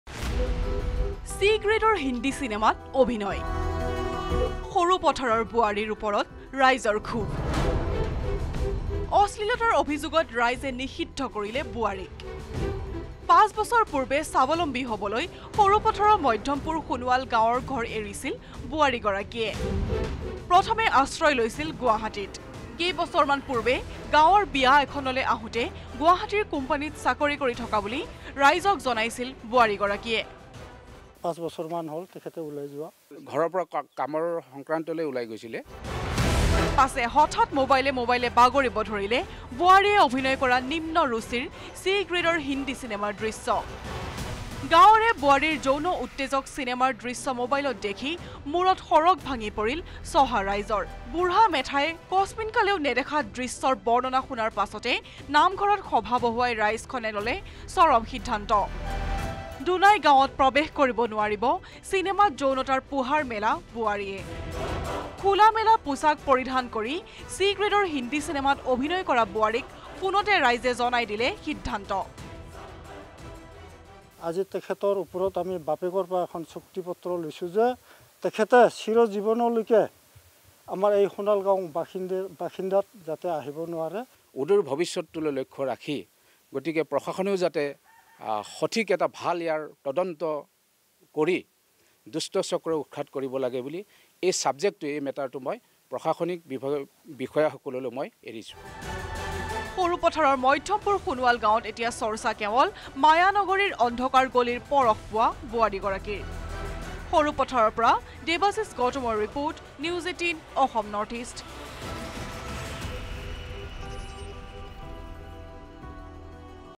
Sarupathar News : সৰুপথাৰৰ বোৱাৰীৰ ওপৰত ৰাইজৰ ক্ষোভ
Sarupathar News | Jorhat ৰ পৰ্ণ ভিডিঅ' কাণ্ডৰ পিচত এইবাৰ তোলপাৰ লগাইছে গোলাঘাট জিলাৰ সৰুপথাৰৰ সীমান্তৱৰ্তী মধ্যমপুৰ সোণোৱাল গাঁও৷